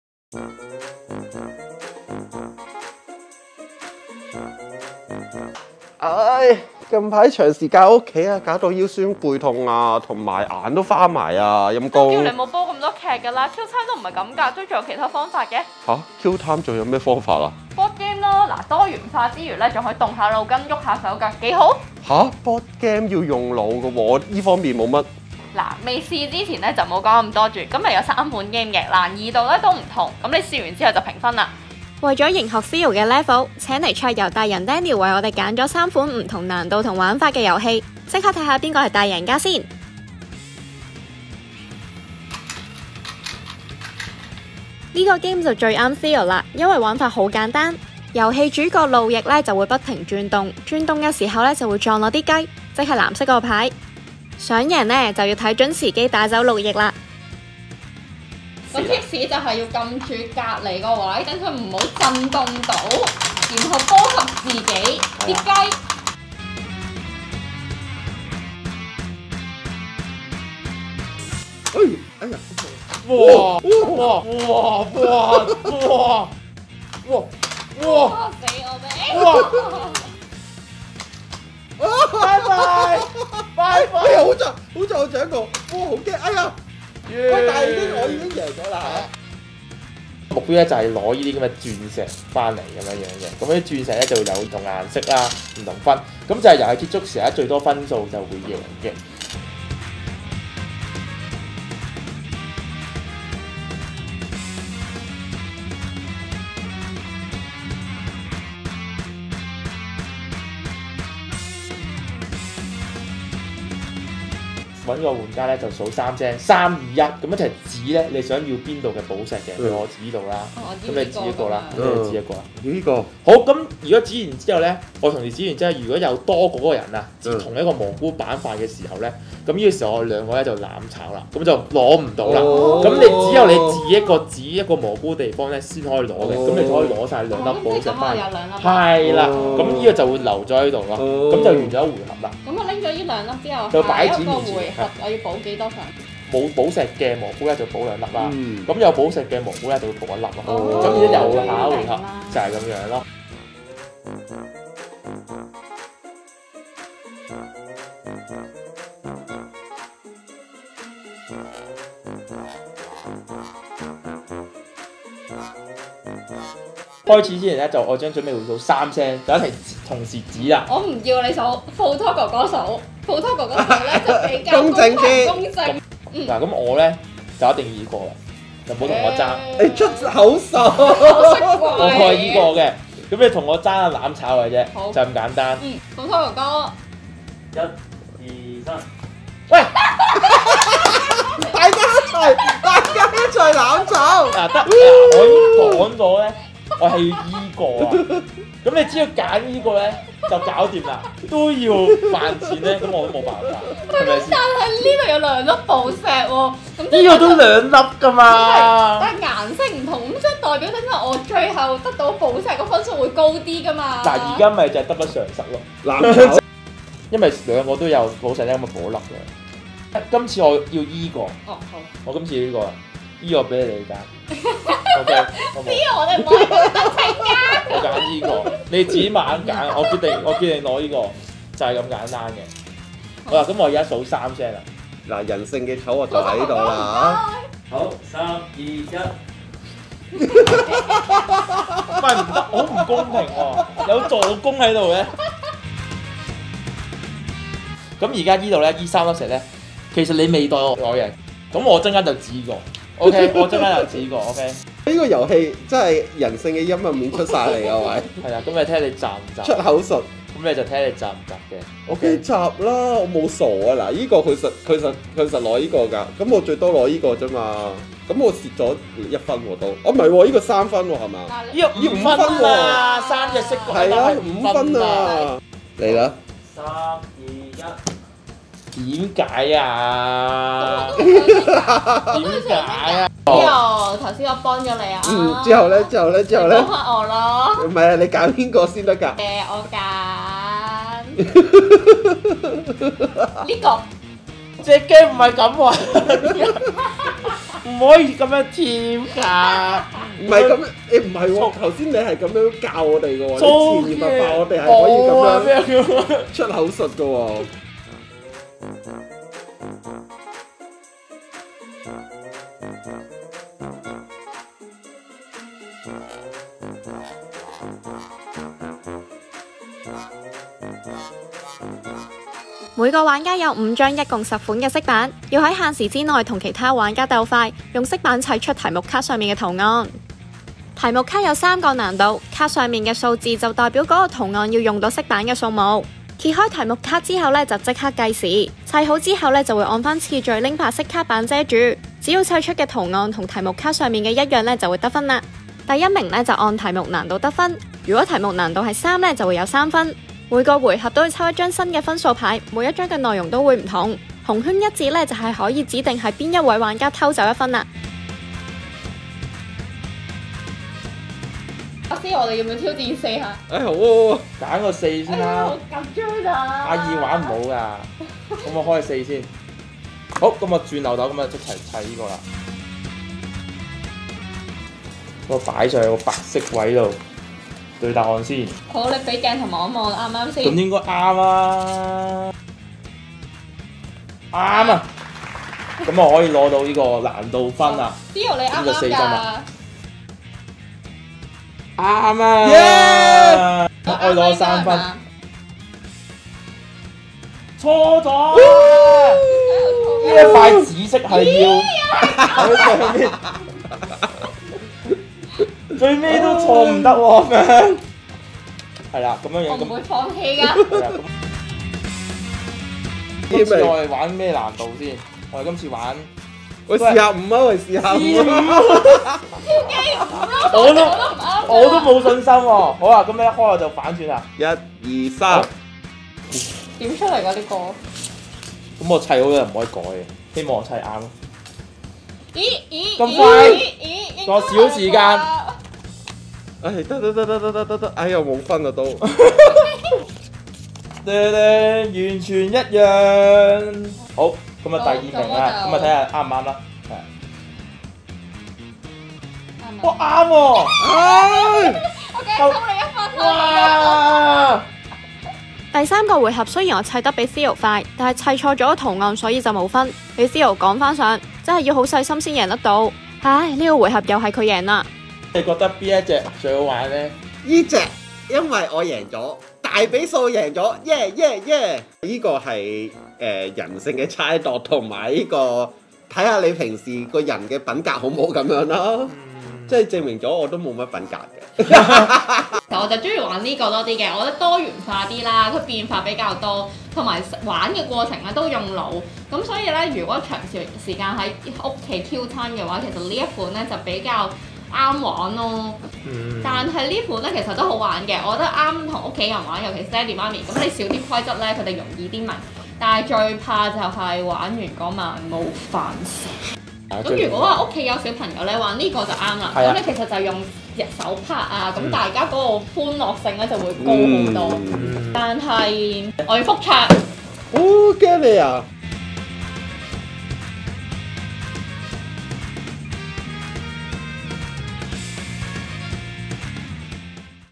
【桌遊香港】KillTime必備！編輯室推介3款桌遊 考反應仲賭埋友情？
唔少朋友為kill time都拎返大富翁、UNO出嚟，其實仲有其他桌遊選擇㗎！今次介紹呢3款玩法、難度都唔同，其中兩款用腦得嚟又唔會太講技巧，最啱同屋企人玩，旨在唔使花太多時間講解玩法。但其中一款分分鐘玩到反面，建議大家晚飯後先玩，咁就唔怕媽咪嬲豬唔煮飯喇！想知點解就睇片喇～

【INFO】
1. 新翻滾路易
參考價錢：$250

2. 寶石獵人
參考價錢：$175

3. 五彩圖層
參考價錢：$210

銷售點：棋間限定 - All On Board HK
地址：油麻地上海街 393 號 2 樓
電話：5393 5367
營業時間：星期一至四 2pm-10:30pm，星期五至日、公眾假期及前夕 2pm-12:30am
全方位緊貼《U Magazine》社交平台